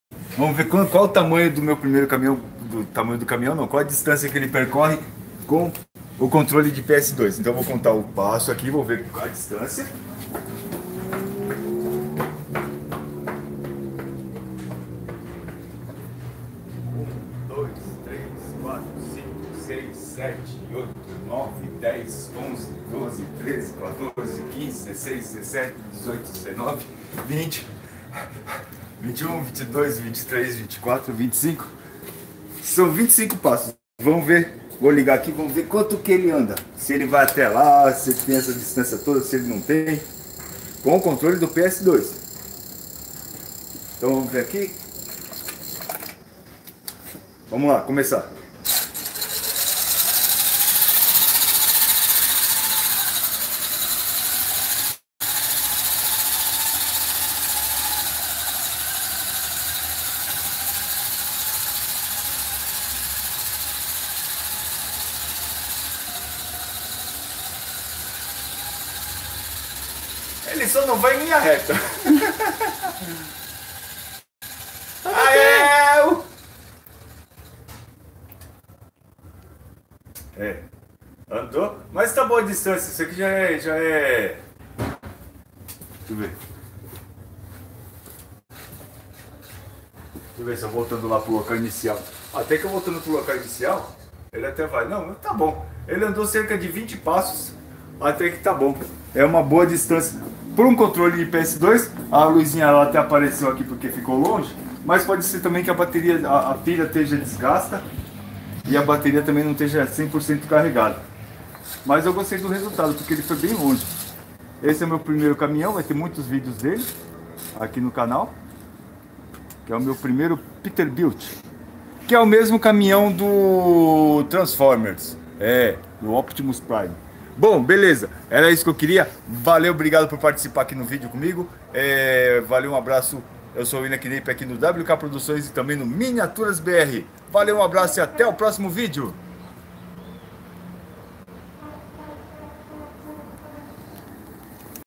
Caminhão peterbilt 1/14 Teste de distância
Nessa pequena live eu fiz um teste de distância do caminhão peterbilt 1/14 que estou fazendo

Se você gostou desse vídeo, deixa o seu like e se gosta do conteúdo, se inscreve aqui no canal